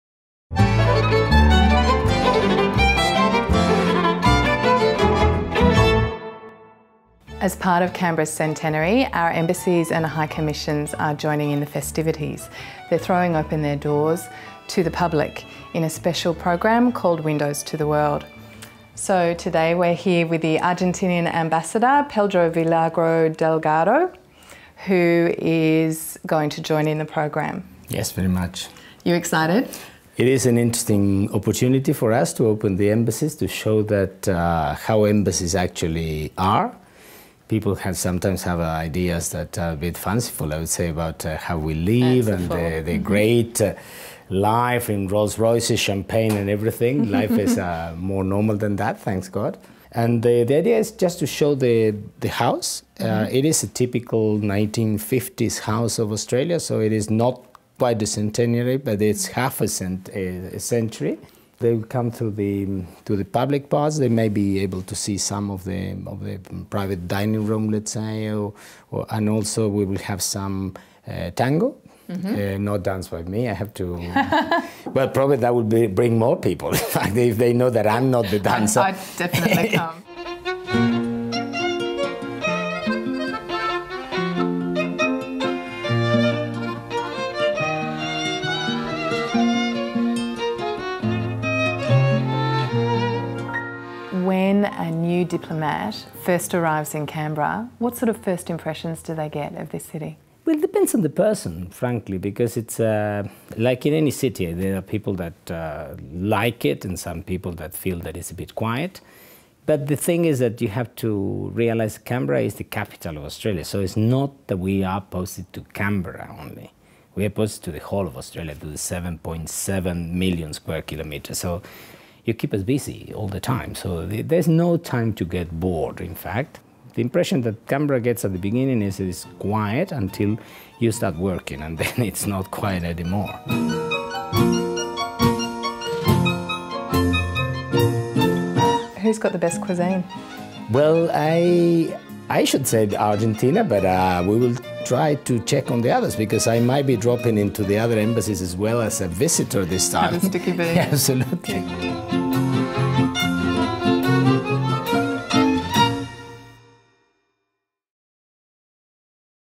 Windows to the World - Argentina
Join Emma Macdonald for a quick glimpse of the Argentinian ambassador's residence in Canberra. Pedro Villagra Delgado shows us what's in store when he opens his house for "Windows to the World" on September 21.